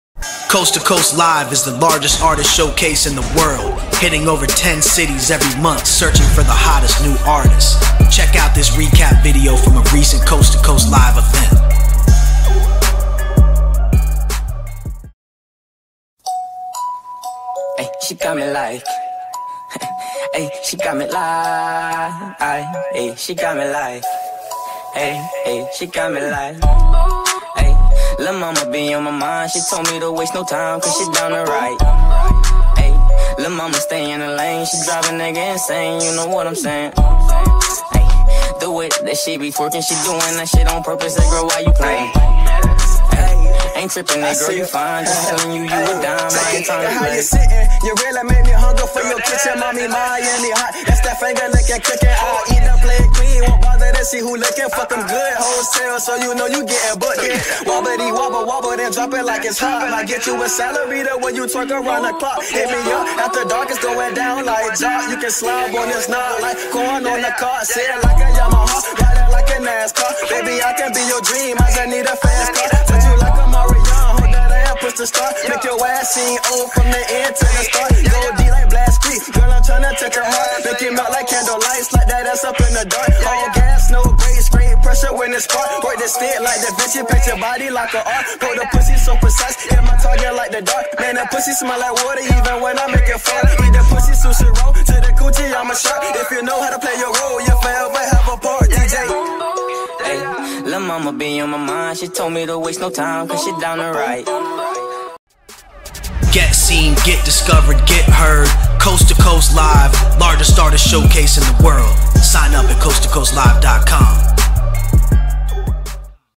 Recap for Coast 2 Coast LIVE | Miami Edition 12/20/18
Showcase Artists
Stonie King
OG Hamm
JWOOD X PROPHECY
Bossanova
Guy Ferz
Dxvice
Llama Dios Rio
TFM~GeminiTwin$
T_wac
 SUPA DADE/J-RAWTHADON/SBGSHOEBOXGANG/COAST2COAST/REVERBNATION
Golden Milk
Most Wanted

Featured Artists
Hero
SavageLife Gbaby
Soto
Marcus Gnosis

DJ Seizure - Huge Music/Warner Bros Official DJ
Coast 2 Coast Mixtape DJ''''''''s
Cool Running DJ''''s
Hazardis Soundz - Producer for NORE, 2Chainz, Fat Joe, Meek Mill, Raekwon, Busta Rhymes, M.O.P, Gunplay and More

Thursday, December 20, 2018 9:00 PM